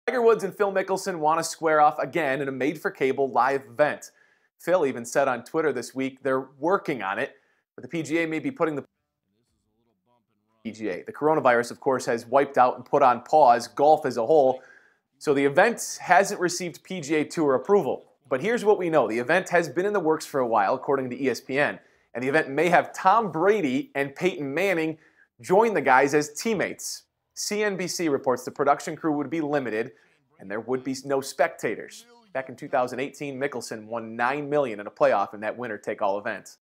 Phil Mickelson, Tiger Woods may face off on cable with Tom Brady, Peyton Mannin
Phil Mickelson, Tiger Woods may face off on cable with Tom Brady, Peyton Mannin. Brad Galli has more.